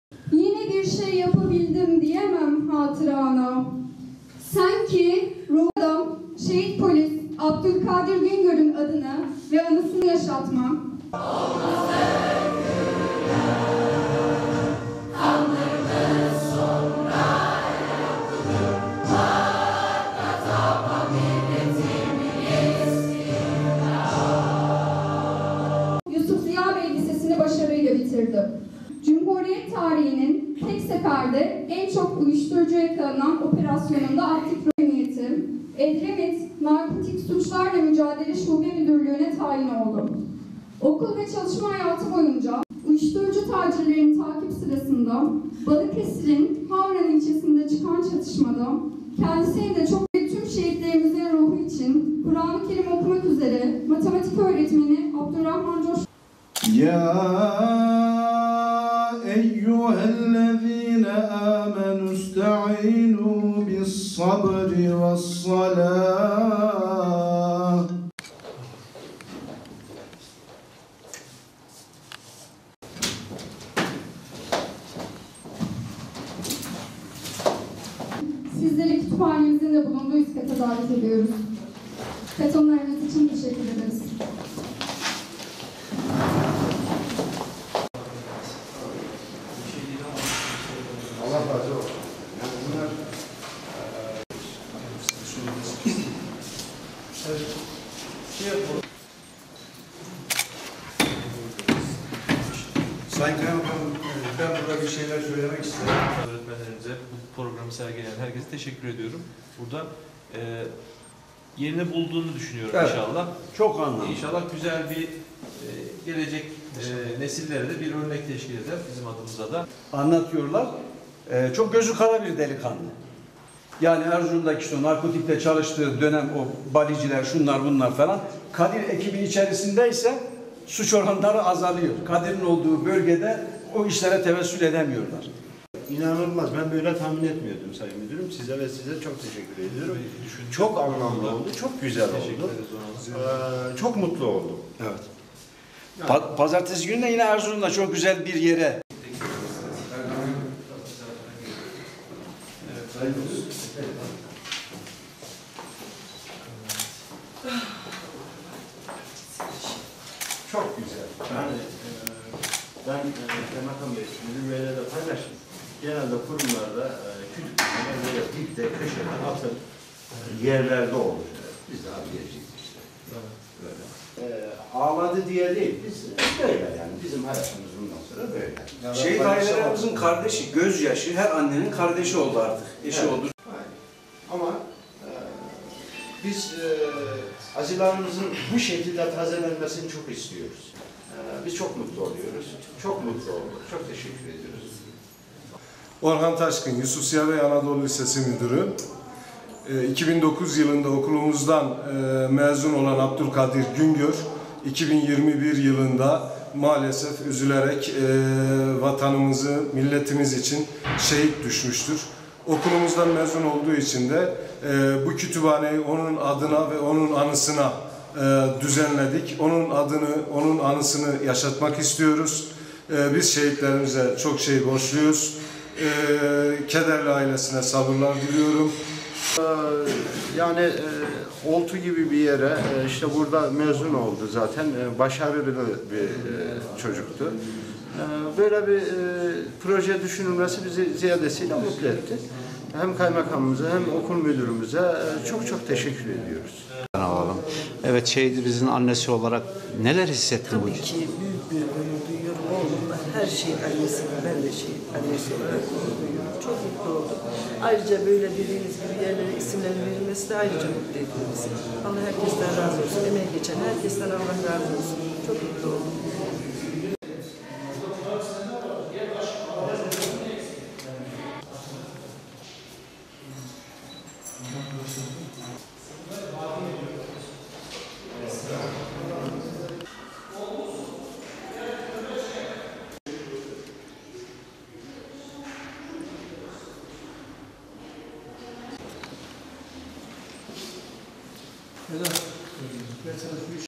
Şehit polis Abdulkadir Güngör’ün ismi kütüphanede yaşatılacak
Balıkesir’de kovaladığı şüphelilerin açtığı ateş sonucu yaralanan ve kaldırıldığı hastanede şehit olan polis memuru Abdulkadir Güngör’ün ismi 2009 yılında mezun olduğu lisede açılan kütüphaneye verildi.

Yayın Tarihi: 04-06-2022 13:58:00